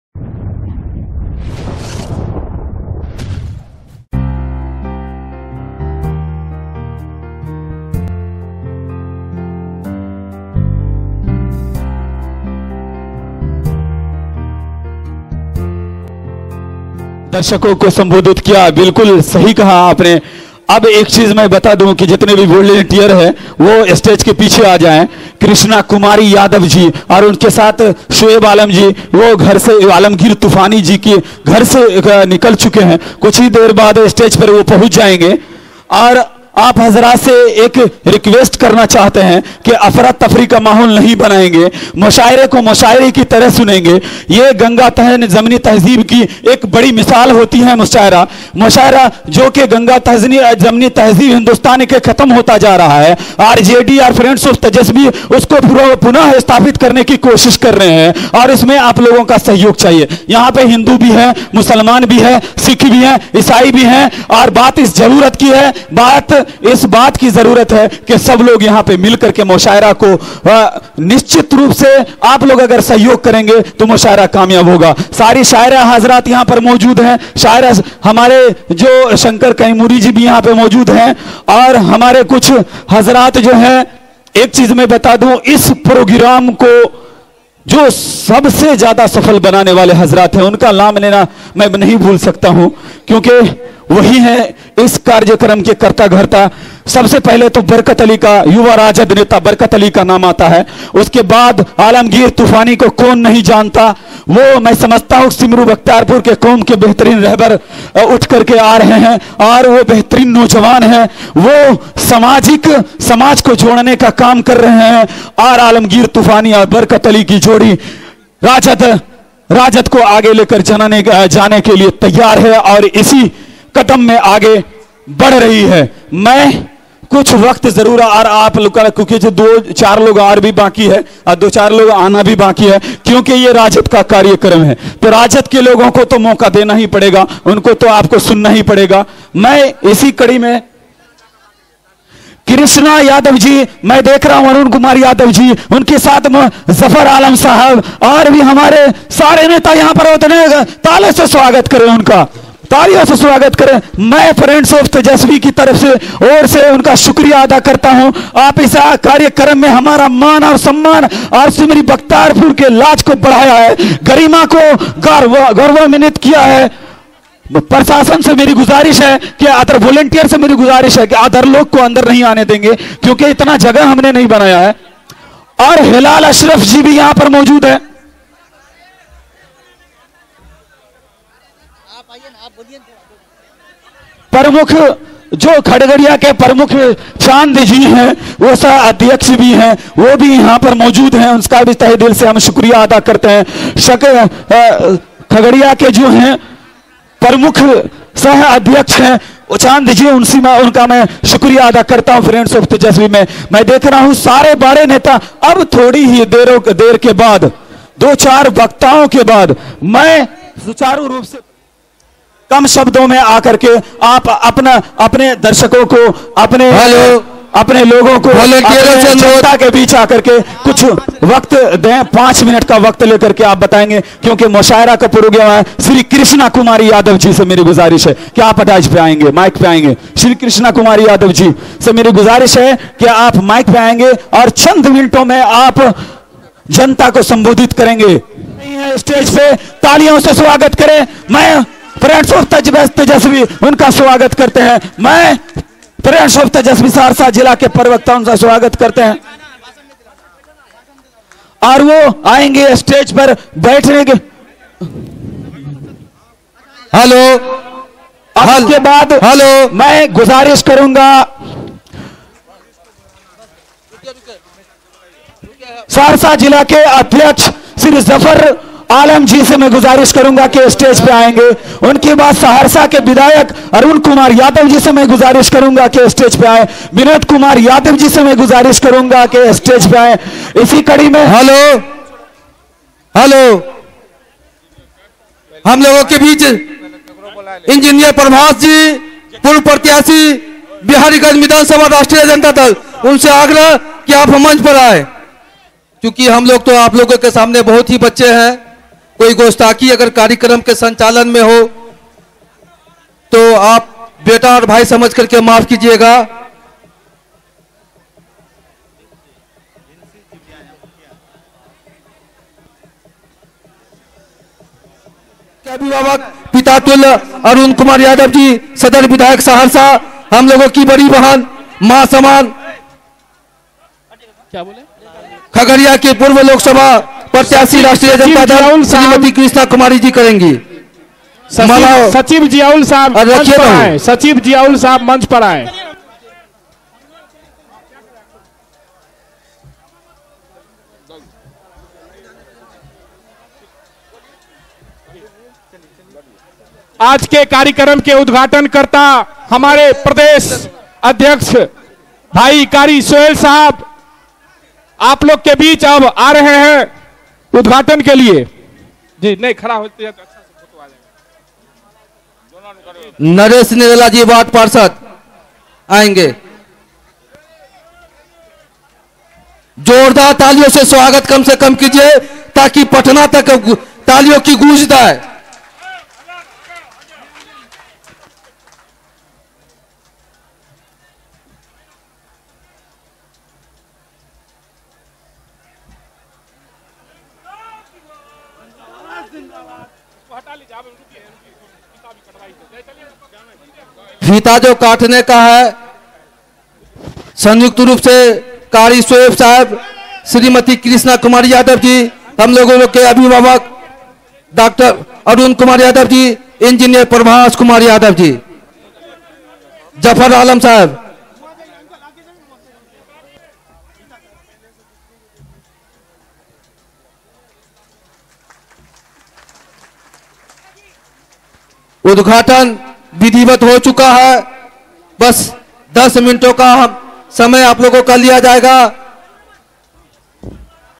Iftetahi Taqreeb, Bakhtiyarpur Bihar Mushaira 2018, Mushaira Media
Iftetahi Taqreeb
All India Mushaira, Simri, Bakhtiyarpur, Bihar
Organized By: FRIENDS OF TEJASWI
Mushaira President & Convenor: Janaab Barkat Ali Saahab
Mushaira Co-Ordinator: Janaab Taha Siddiqui Saahab
Held On 23 Sep 2018